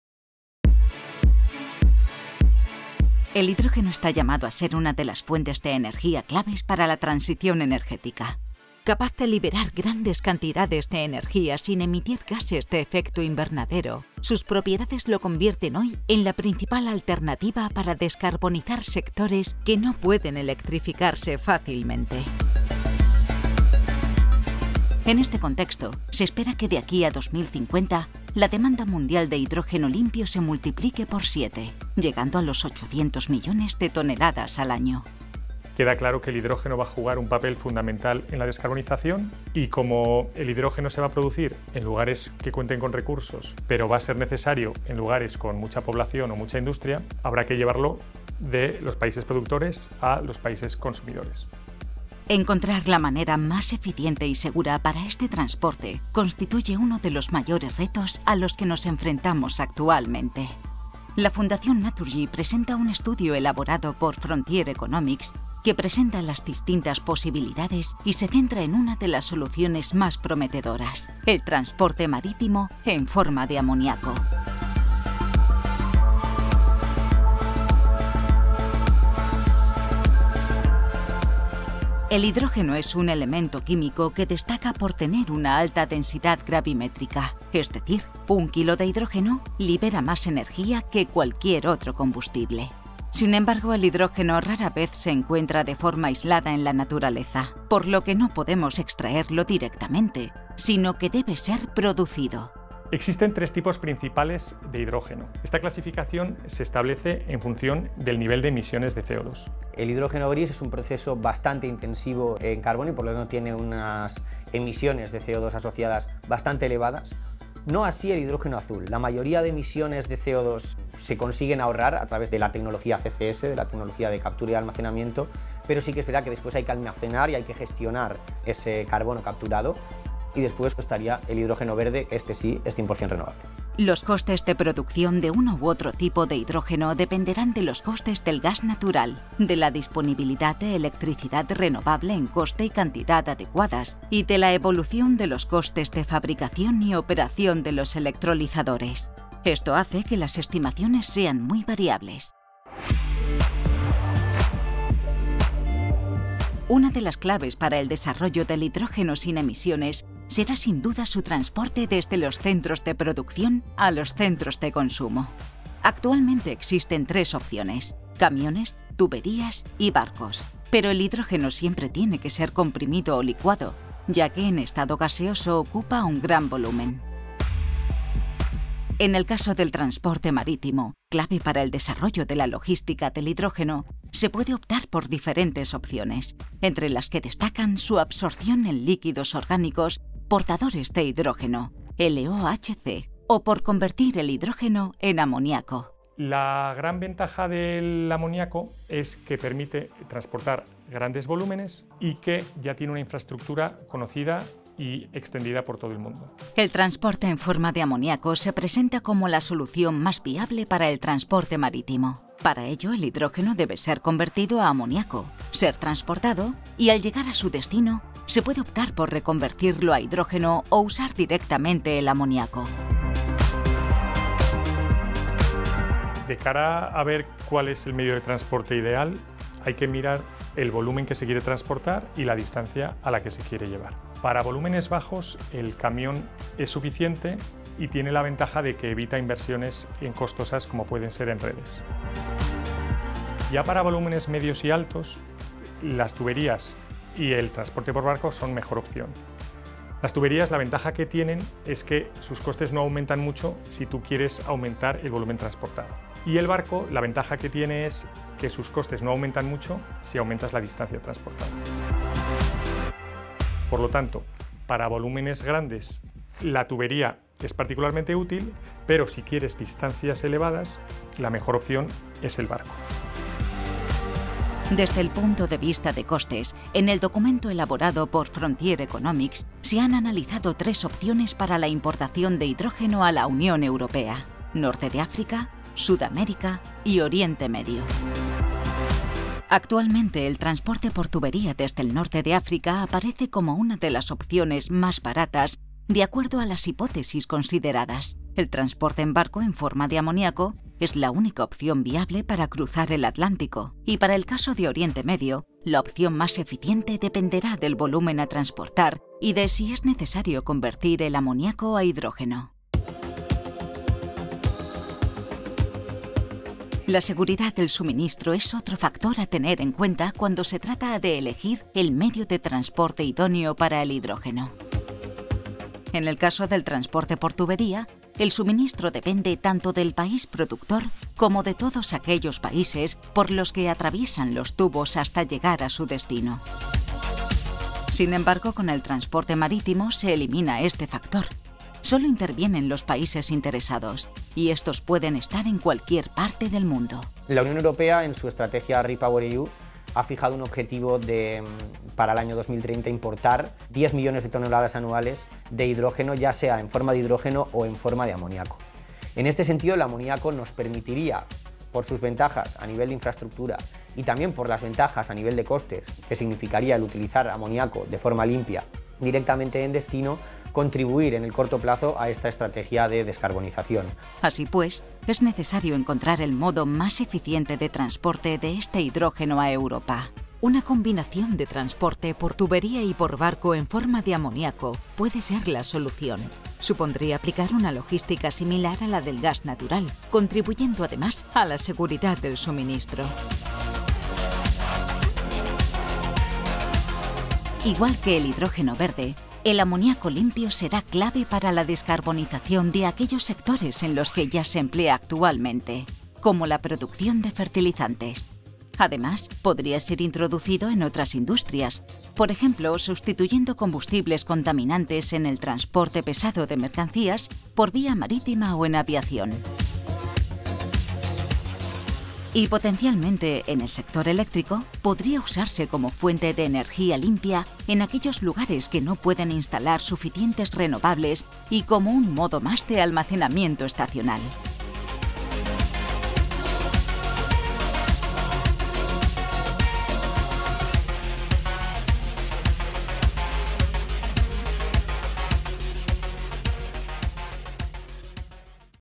El transporte de hidrógeno: la importancia del amoniaco
Este vídeo es un resumen del estudio “El transporte de hidrógeno: la importancia del amoniaco”, elaborado por consultora Frontier Economics y editado por la Fundación Naturgy.

El hidrógeno está llamado a ser una de las fuentes de energía claves para la transición energética. Capaz de liberar grandes cantidades de energía sin emitir gases de efecto invernadero, sus propiedades lo convierten hoy en la principal alternativa para descarbonizar sectores que no pueden usar otras energías renovables.

En este contexto, se espera que de aquí a 2050 la demanda mundial de hidrógeno limpio se multiplique por 7, llegando a los 800 millones de toneladas al año.

Estas grandes cantidades de hidrógeno limpio, que pueden representar hasta una cuarta parte de la energía mundial, van a tener que ser transportadas internacionalmente desde países con elevada capacidad de producción a países altamente poblados e industrializados.
El hidrógeno es un elemento químico que destaca por tener una alta densidad gravimétrica, es decir, un kilo de hidrógeno libera más energía que cualquier otro combustible.

Sin embargo, el hidrógeno rara vez se encuentra de forma aislada en la naturaleza por lo que no podemos extraerlo directamente, sino que debe ser “producido”. Los costes de producción de uno u otro tipo de hidrógeno dependerán de los costes del gas natural; de la disponibilidad de electricidad renovable en coste y cantidad adecuadas; y de la evolución de los costes de fabricación y operación de los electrolizadores. Esto hace que las estimaciones sean muy variables.

Una de las claves para el desarrollo del hidrógeno sin emisiones será sin duda su transporte desde los centros de producción a los centros de consumo. Actualmente existen tres opciones: camiones, tuberías y barcos. Pero el hidrógeno siempre tiene que ser comprimido o licuado, ya que en estado gaseoso, ocupa un gran volumen.

En el caso del transporte marítimo, clave para el desarrollo de la logística del hidrógeno, se puede optar por diversas opciones, entre las que destacan su absorción en líquidos orgánicos portadores de hidrógeno (LOHC), o por convertir el hidrógeno en amoniaco. El transporte en forma de amoniaco se presenta como la solución más viable para el transporte marítimo.

Para ello, el hidrógeno debe ser convertido a amoniaco, ser transportado y, al llegar a su destino, se puede optar por reconvertirlo a hidrógeno o usar directamente el amoniaco.

Desde el punto de vista de costes, en el documento elaborado por Frontier Economics se han analizado tres opciones para la importación de hidrógeno a la UE: Norte de África, Sudamérica y Oriente Medio.

Actualmente, el transporte por tubería desde el norte de África aparece como una de las opciones más baratas de acuerdo a las hipótesis consideradas; el transporte en barco en forma de amoníaco es la única opción viable para cruzar el Atlántico; y para el caso de Oriente Medio, la opción más eficiente dependerá del volumen a transportar y de si es necesario reconvertir el amoniaco a hidrógeno.

Además, hay que considerar que conectar Europa con esta región, construyendo hidroductos a través de varios países, es una vía de mucha complejidad.